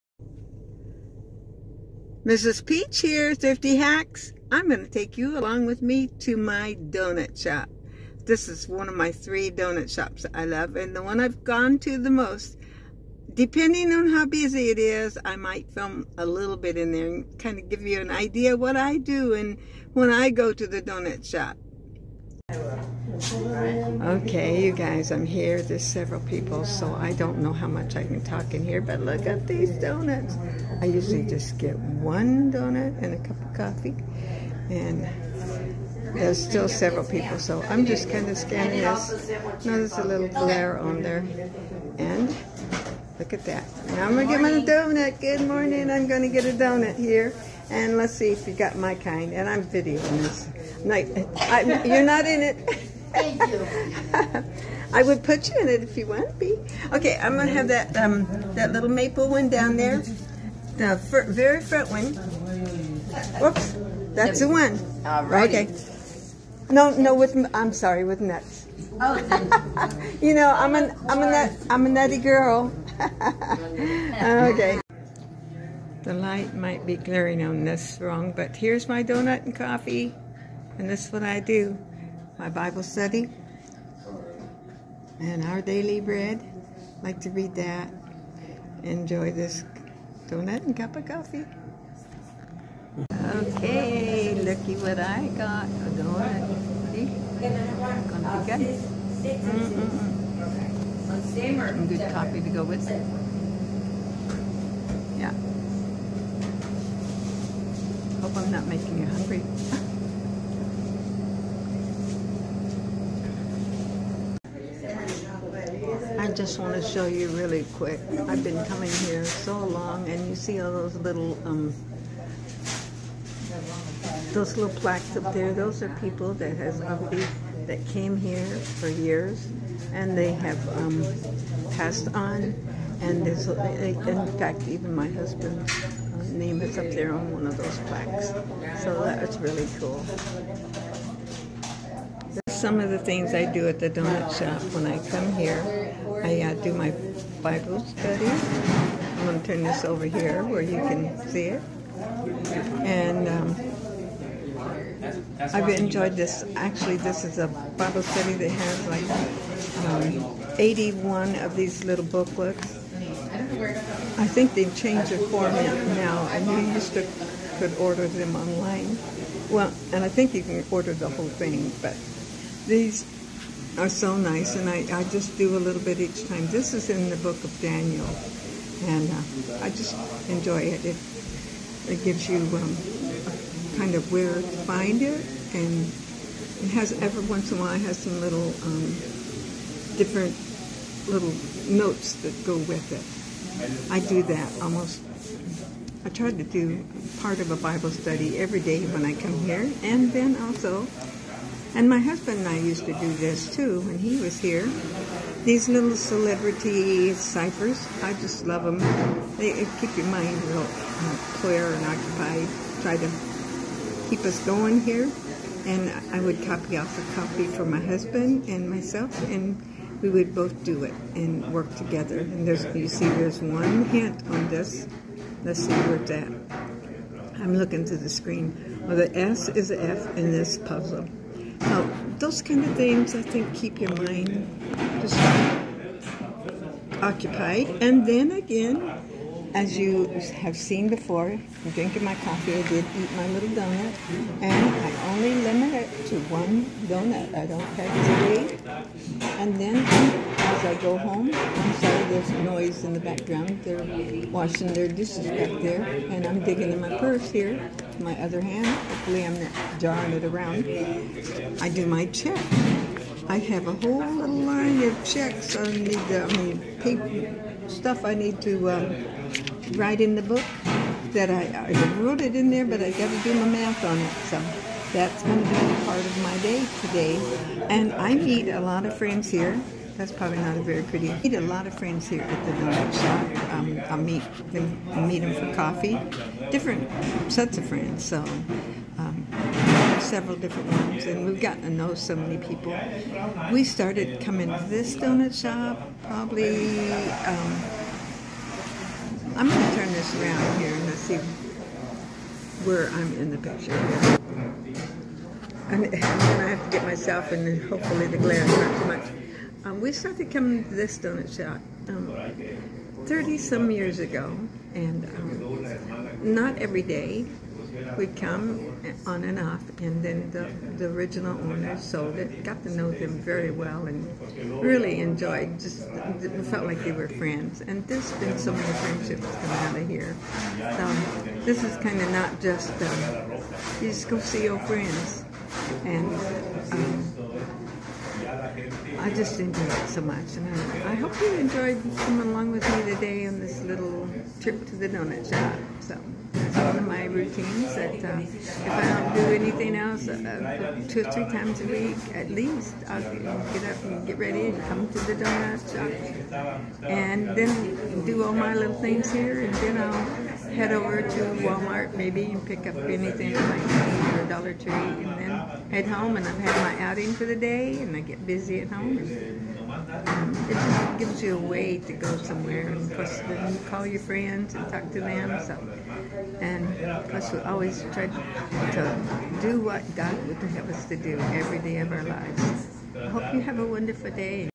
my DONUT time 🍩... visit with me!
Enjoy visiting with me as I'm at the donut shop, I have a little routine of what I do, I hope you can hear me, at times it was a bit noisy.(after I listened to this on the big screen I realized how loud the back ground was)...I want to tell you about the  "Christian Worker Bible Study Series"..Global Youth Evangelism, Orland, Ca. if anyone is interested.